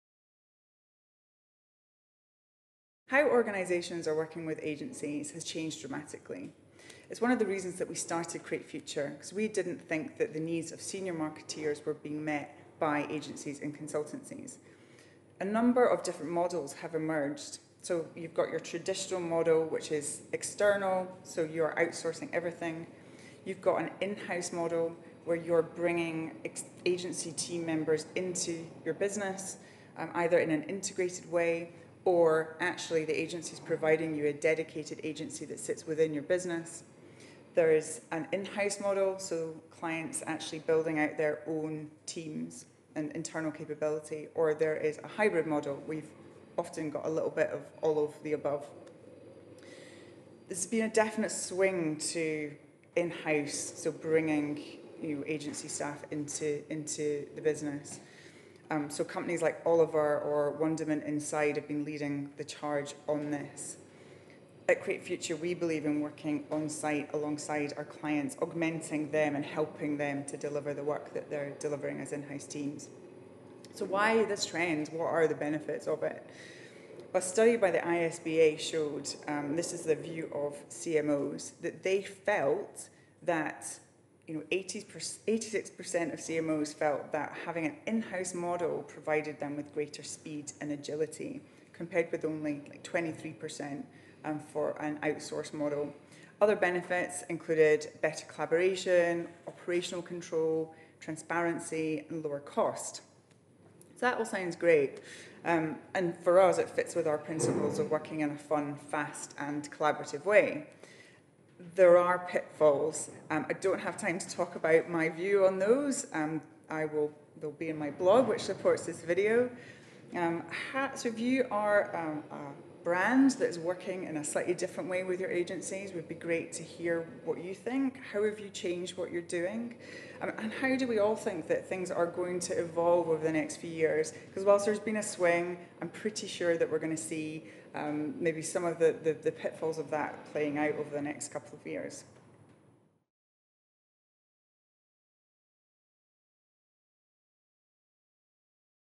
Is the client - agency relationship changing?
New models are emerging all the time - Jessica looks at the pros, cons and some of the considerations that often get overlooked.

To read more on Jessica's thoughts - Check out the blog